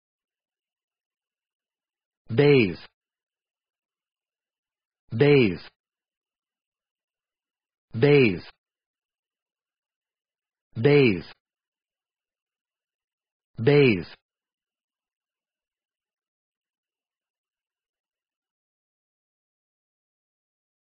How to pronounce bathe
bathe American English pronunciation. How to pronounce bathe correctly. How to say bathe in proper American English.